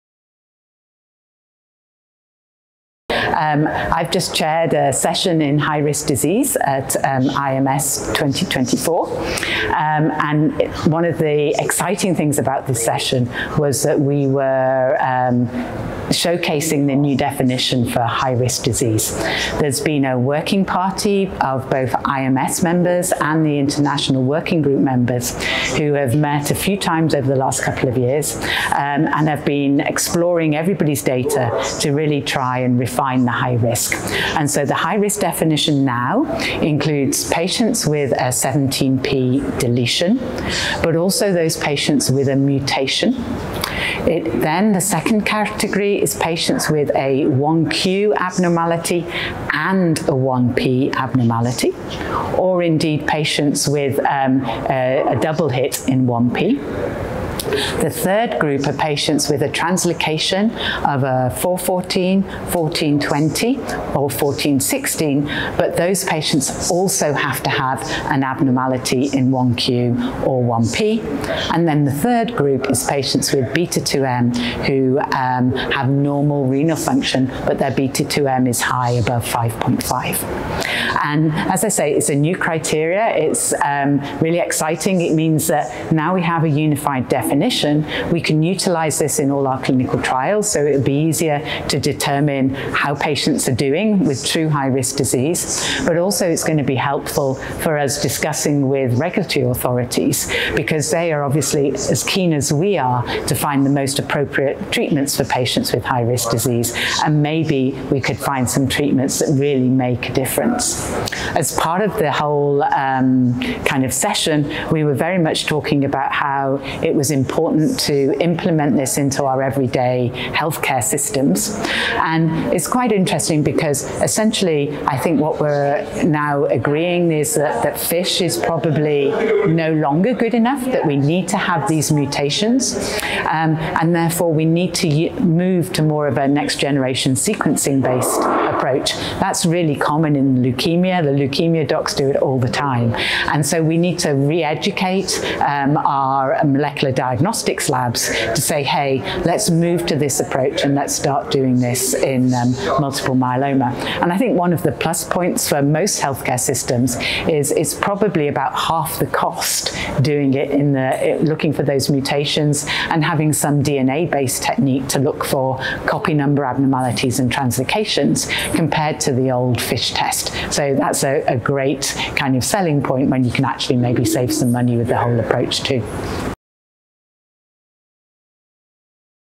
The new criteria for defining high-risk multiple myeloma
Faith Davies, MBBCh, MRCP, MD, FRCPath, NYU Langone Medical Center, New York, NY, discusses the recent updates to the criteria for identifying patients with high-risk multiple myeloma (HRMM), outlining which groups of patients are now considered to be high-risk. Dr Davies also highlights that moving from fluorescence in situ hybridization (FISH) testing to a next-generation sequencing (NGS)-based approach will likely confer an economic benefit to healthcare systems. This interview took place at the 21st International Myeloma Society (IMS) Annual Meeting, held in Rio de Janeiro, Brazil.